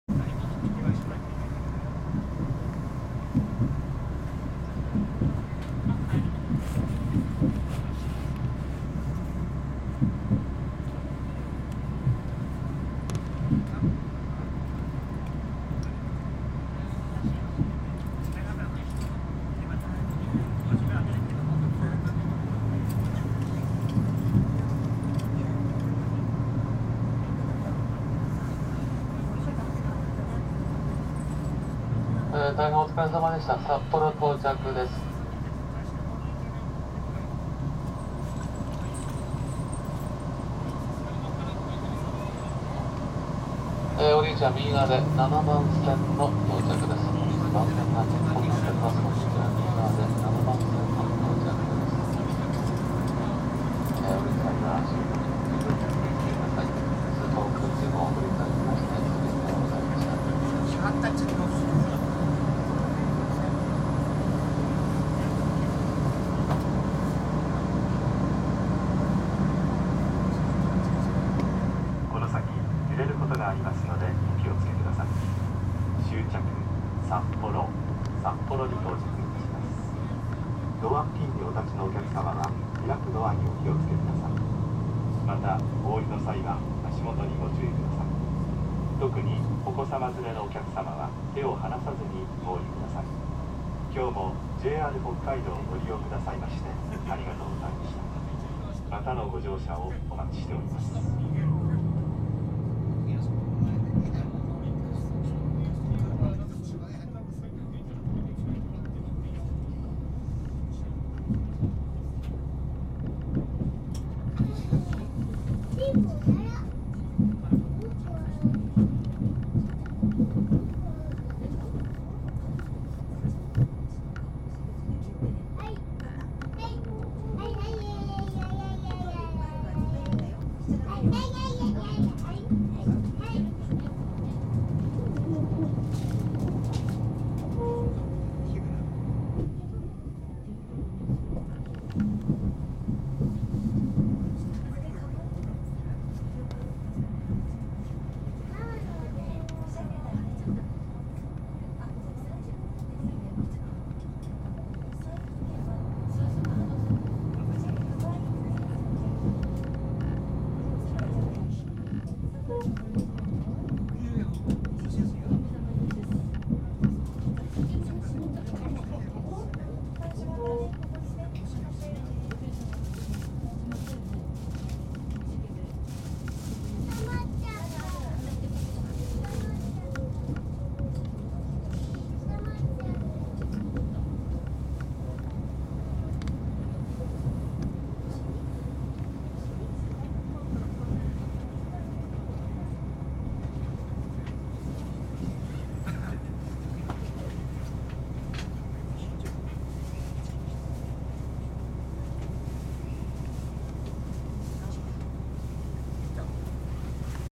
高架軌道の線路が雪に埋もれた札幌駅に到着する特急スーパー北斗キハ261系の車窓
高架軌道の線路が雪に埋もれた札幌駅に到着する特急スーパー北斗キハ261系の車窓です。
札幌の街が深い雪に覆われた厳寒の真冬の千歳線を、下り列車の特急スーパー北斗キハ261系が走行していき、車窓には大通公園のテレビ塔やサッポロビール工場跡のモニュメントなどが見えてきます。
高架区間となっている線路軌道は鉄路を見ることができないほど深い雪に覆われており、多数の線路が並んでいるはずの札幌駅構内に入ると高架軌道が広い雪原のようになってしまい、様々な列車が発着して賑いのある終点の札幌駅ホームに到着していきます。